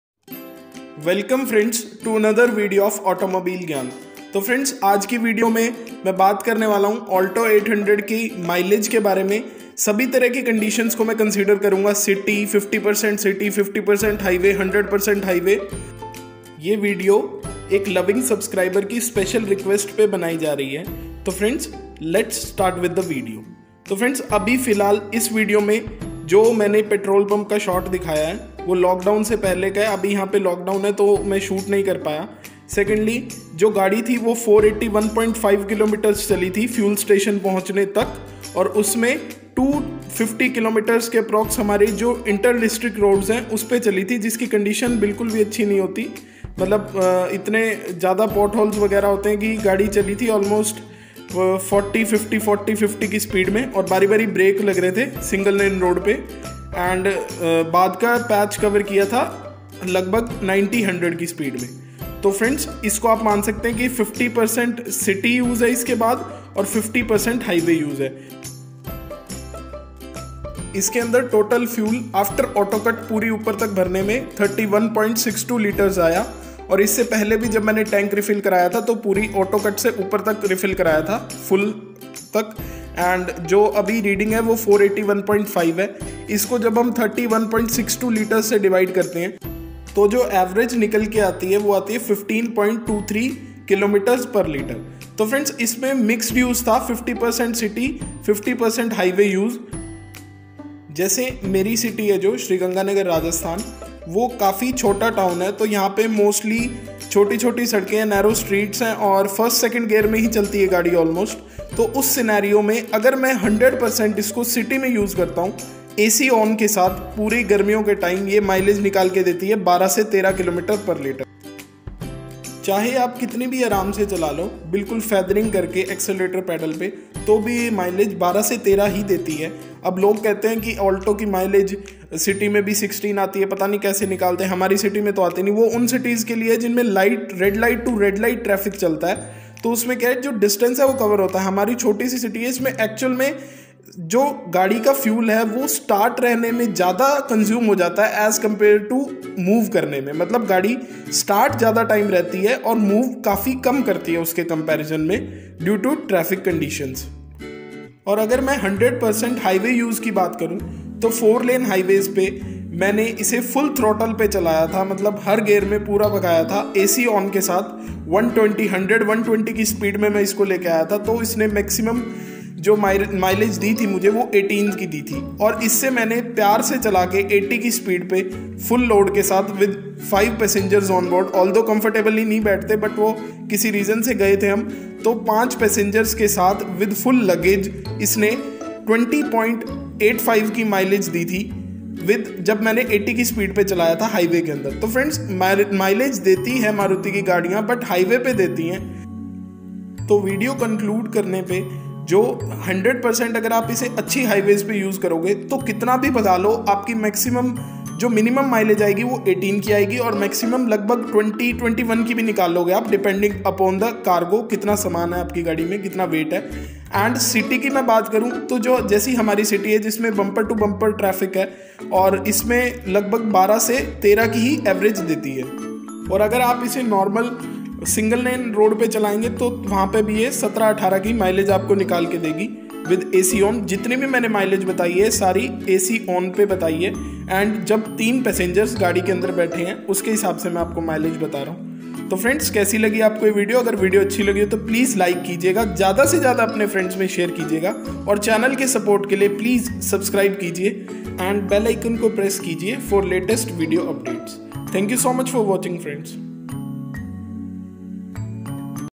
Alto 800 Real Condition Mileage Test | Traffic me सच में इतनी कम Avg. ही देती है❓Highway | City🔥
In This Video I have shared complete detailed information of Alto 800 Mileage Test in
 City condition

single lane highway

Four lane highway

Controlled situation

Full Acceleration

If for any reason if you like the video please give it a thumbs up 👍

To share ownership review of your car Please do mail your information to me at

Link 🔗 for my previous videos on

If You are still reading this then don't forget to

and
Do like
and comments